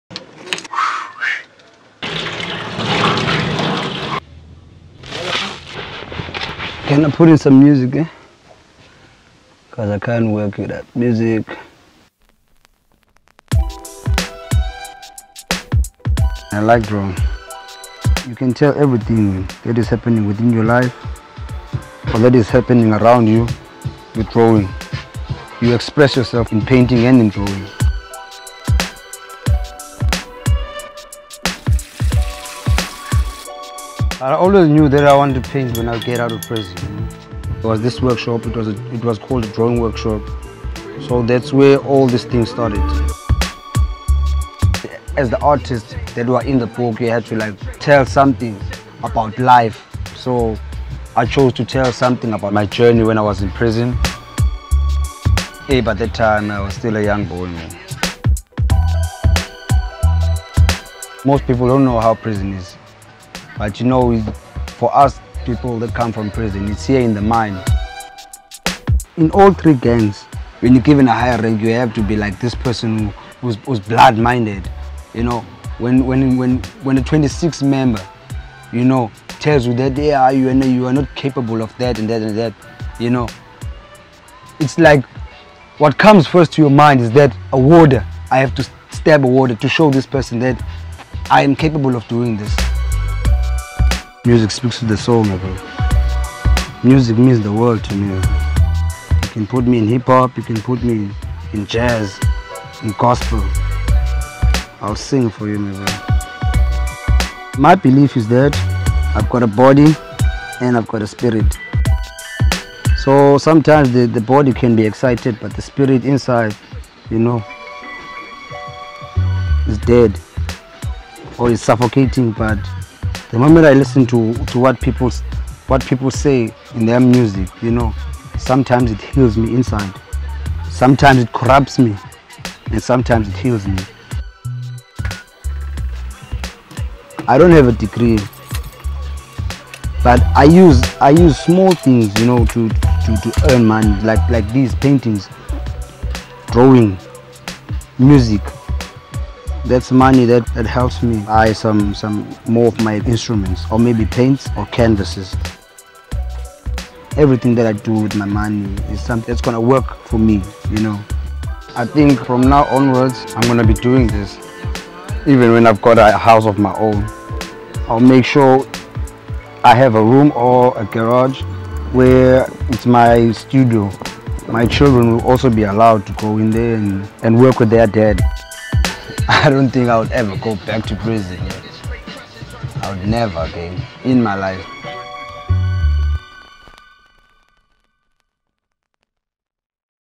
Ex inmate reflects on his prison time through comics
Loyiso Botha, formerly incarcerated in Pollsmoor and later in Drakenstein Correctional Centres, uses art and music to reflect on a life of crime.

Produced by: Thomas Holder